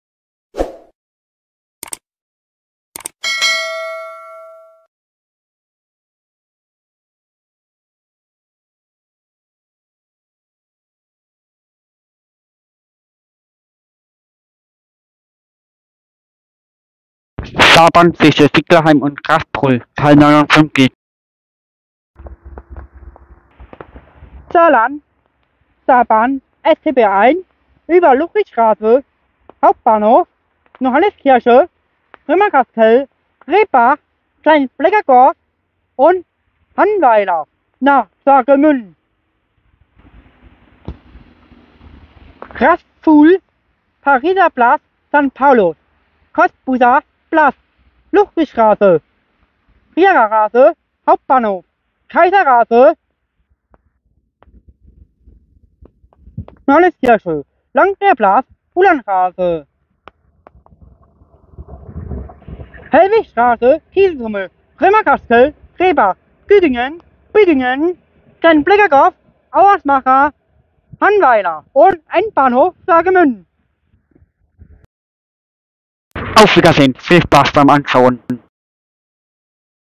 Saarbahn zwischen Siedlerheim & Rastpfuhl Teil 59 | Job🎫 | @Trainspotter.Manuel.2025 | Vlog 1.164
Saarbahn zwischen Siedlerheim  £ Rastpfuhl  #59
Viedeonummer (1️⃣1️⃣6️⃣4️⃣)

Viedeoaufnehmen: im Oktober 2022

Mit dem Jobtickets für Saarbrücken

📍Saarbrücken-Malstatt
🚏Siedlerheim

1. 🚋 Sarreguemines  (1019 | Saarland)

Wagen: 1
Fahezug: 1️⃣0️⃣1️⃣9️⃣
Linie: 1
Ziel: Sarrguemines
Über: Ludwigstraße , Hauptbahnhof  , Johenneskirche

1️⃣0️⃣1️⃣9️⃣: Saarland

Fahrzeugnummer: 1019

1. Fahrzeug
Saarbahn
🚋 STB1
Vom Siedlerheim
Über   Ludwigstraße - Hauptbahnhof  - Johenneskirche -   Römerkastell - Brebach  - Kleinblittersdorf  - Hanweiler
Nach   Sarrguemines
🚏 Rastpfuhl - Pariser Platz
🚋 Siedlerheim 》 Sarrguemines
️1️⃣0️⃣1️⃣9️⃣:  SAARLAND

1. Fahrzug
📍🅰️Siedlerheim 16:00
⬇️Rastpfuhl 16:01
⬇️Pariser Platz/ St. Paulus 16:03
⬇️Cottbuser Platz 16:04
🔽Ludwigstraße 1️⃣6️⃣:0️⃣6️⃣
⬇️Trierer Straße 16:08
🔽Hauptbahnhof 1️⃣6️⃣:1️⃣0️⃣
⬇️Kaiserstraße 16:11
🔽Johanneskirche 1️⃣6️⃣:1️⃣3️⃣
⬇️Landwerplatz 16:14
⬇️Uhlandstraße 16:16
⬇️Hellwigstraße 16:17
⬇️Kieselhumes  ️16:19
🔽Römerkastell 1️⃣6️⃣:2️⃣0️⃣
🔽Brebach 1️⃣6️⃣:2️⃣4️⃣
⬇️Güdingen 16:26
⬇️Bübingen 16:28
🔽Kleinblittersdorf 1️⃣6️⃣:3️⃣1️⃣
⬇️Ausermacher 16:34
🔽Hanweiler Bad Richlingen 1️⃣6️⃣:3️⃣7️⃣
🅱️Sarrguemines 16:40